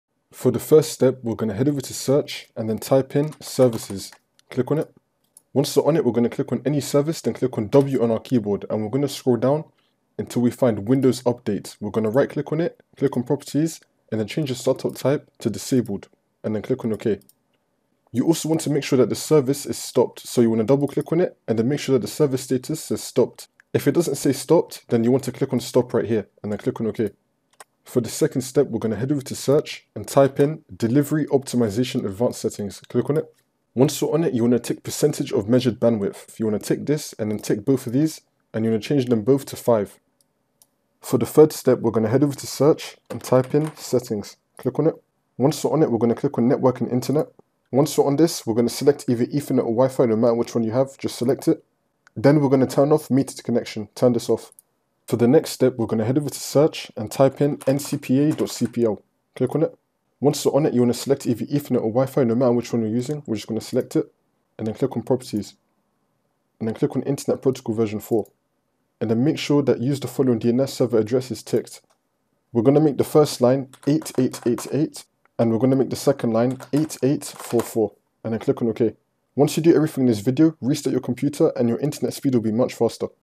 How To Increase Internet Speed On Windows PC & Laptop (2024 Updated)
How To Increase Internet Speed On Windows PC - (2024 Updated)
This video will let you know what all you can do to boost internet speed on Windows 11/10/7 PC and laptop.
Please do not skip any part and follow all the suggestions and changes.

This will fix slow internet issues like while downloading or browsing on browsers.